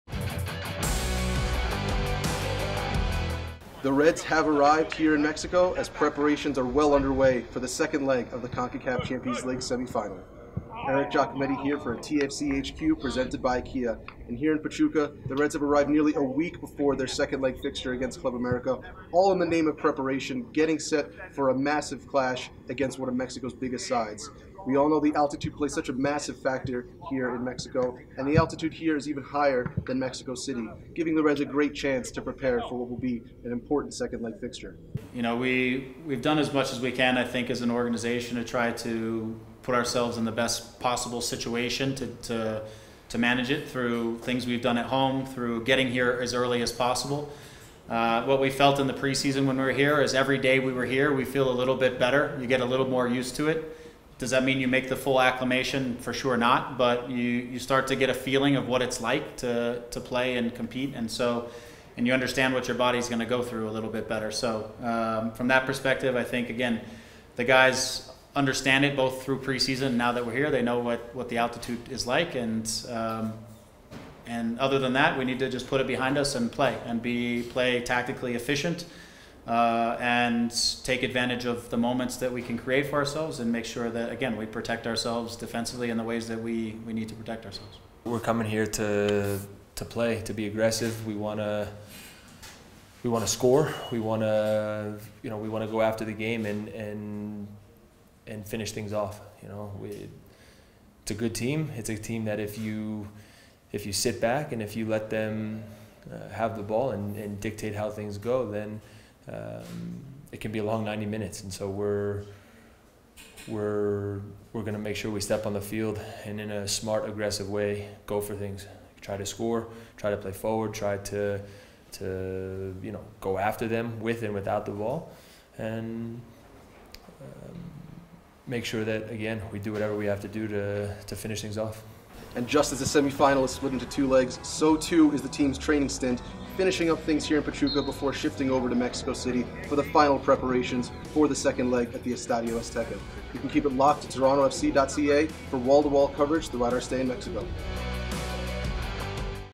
TFC HQ: Early Arrival in Mexico - April 7, 2018
Eric Giacometti has the latest from Toronto FC's training session as the Reds get set ahead of their return leg against Club América.